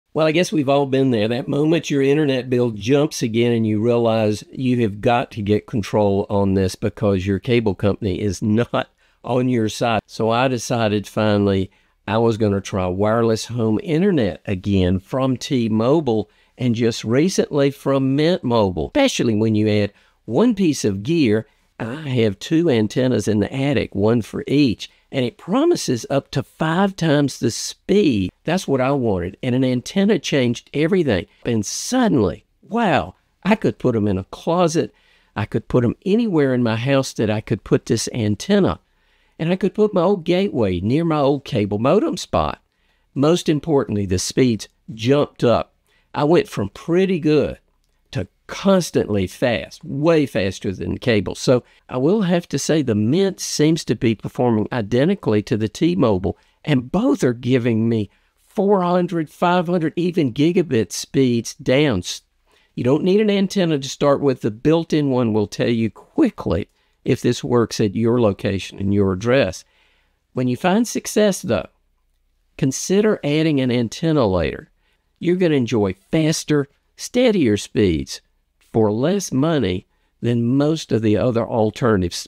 Mint Mobile + T-Mobile vs Cable - The Real Winner
Frustrated with increasing cable bills, we explore the world of `wireless home internet` using T-Mobile and Mint Mobile services. This video details how `fixed wireless access` can provide a reliable `internet connection`, especially when optimized with external antennas. Discover how to significantly boost your `internet speed` and whether `t-mobile home internet` is the perfect solution for your home! 📡